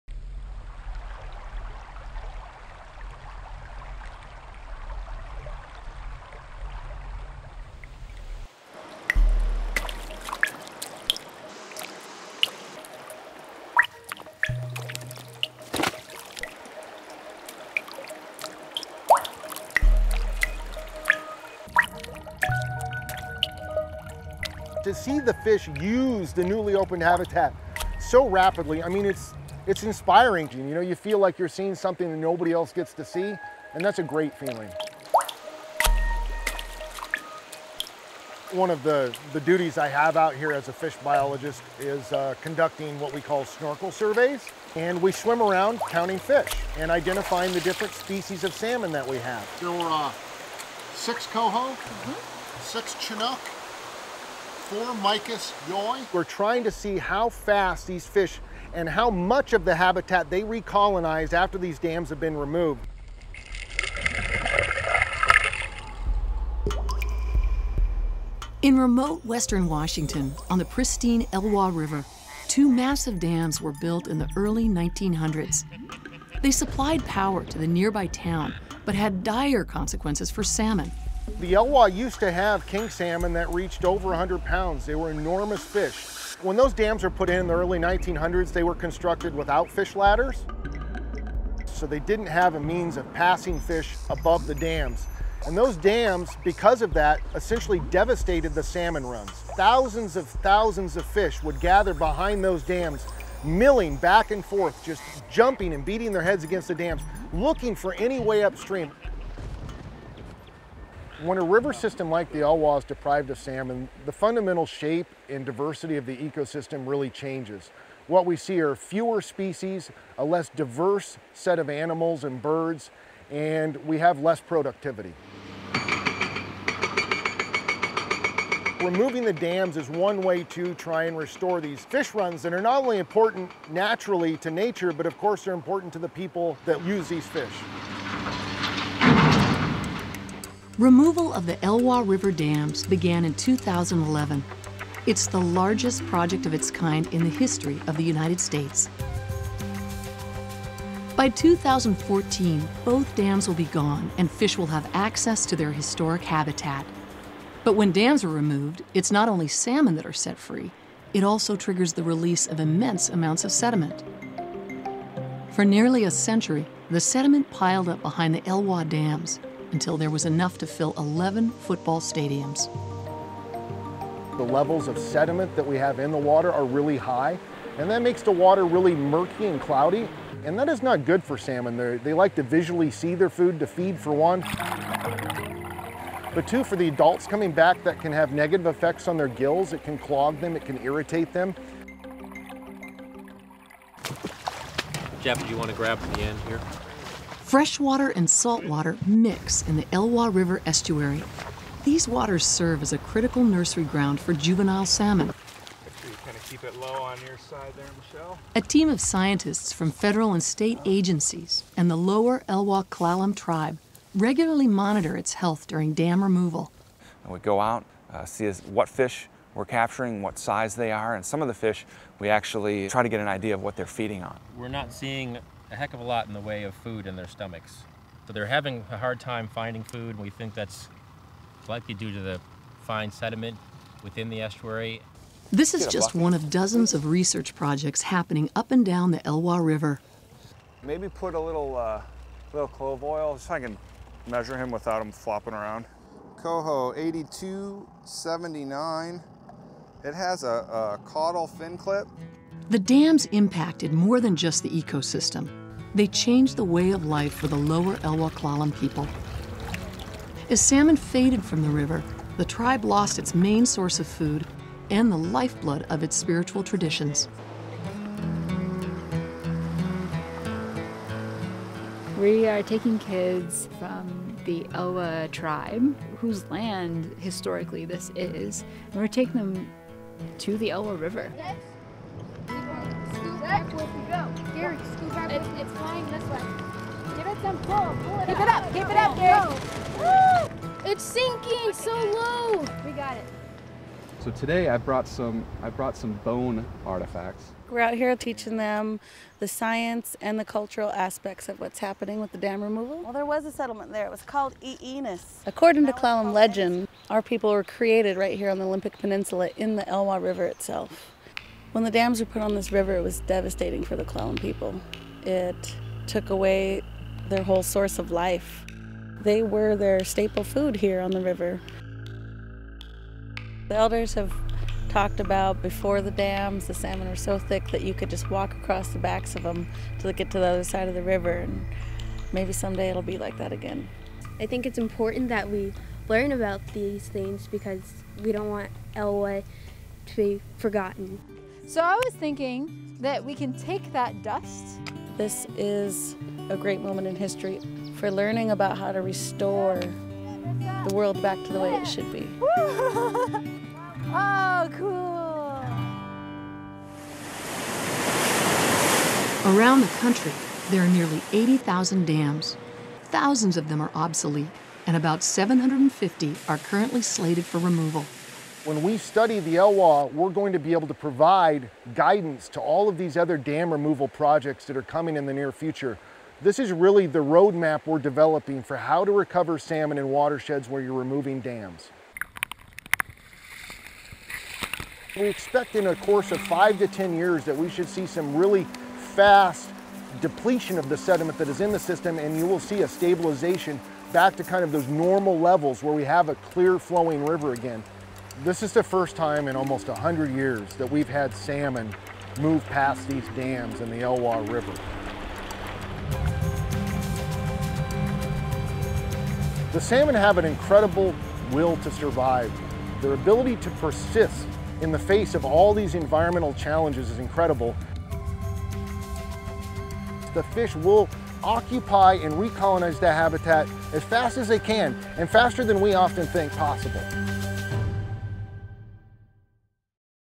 A River Returns
In Washington state, a river once known for its abundant salmon run is getting a second chance. The Elwha River dams, which decimated salmon populations and profoundly altered the ecosystem, are coming down and hopes are high that salmon will return.